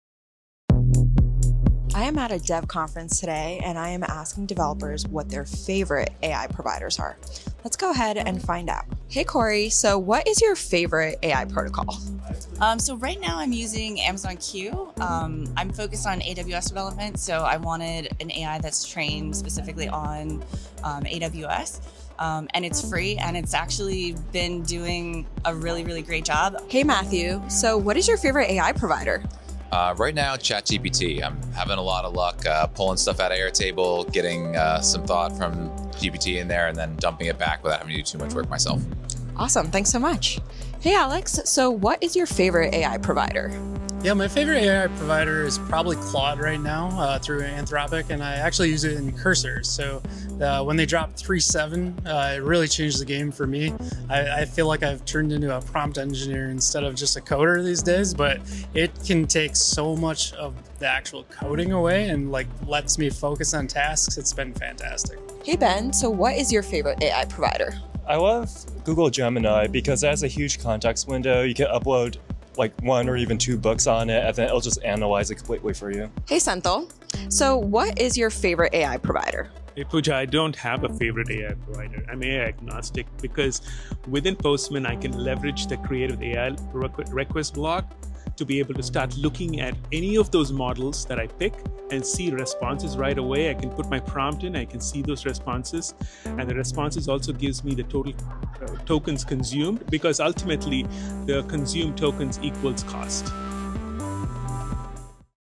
Which AI Provider Do Top Tech Experts Trust?
Pooja Mistry asks industry experts about their favorite AI provider at DevOps Days in Chicago.

Matthew Du Pont. - Sales Executive
Alex Patterson. - Dev Rel Engineer